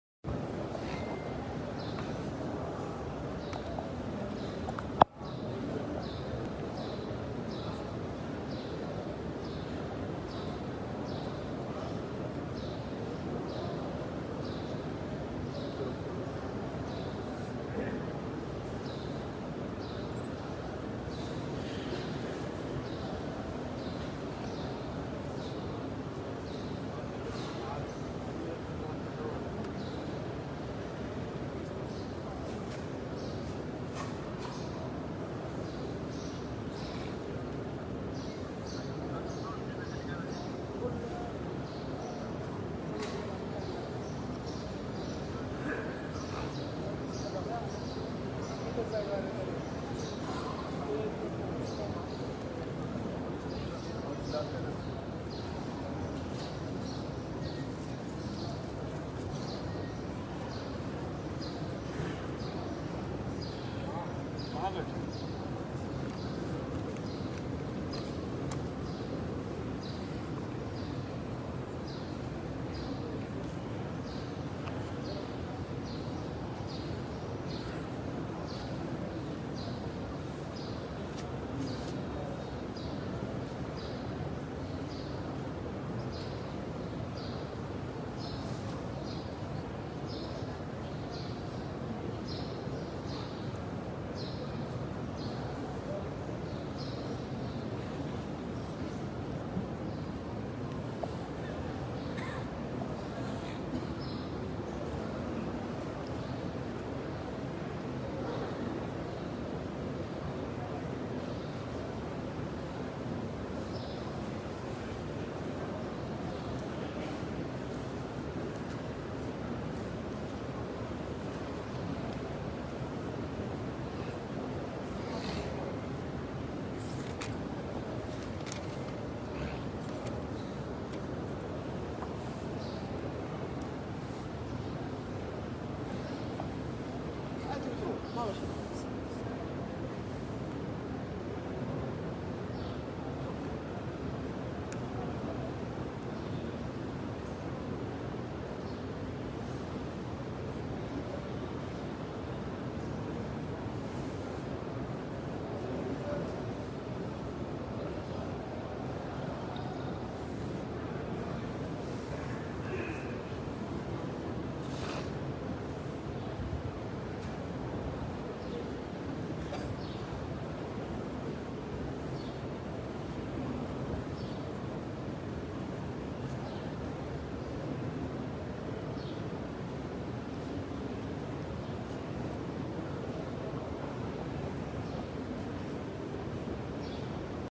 Prayer At Masjid Al Haram Live 2022-মসজিদ আল হারামে নামাজ আদায়।
মসজিদ আল হারামে কিভাবে নামাজ আদায় হয়।
মসজিদ আল হারামে নামাজ পড়ার দৃশ্য।
মসজিদে হারামের ভিতরের দৃশ্য।
Prayer At Masjid Al Haram Live 2022
prayer in masjid al haram is worth
masjid al haram salat
salah in masjid al haram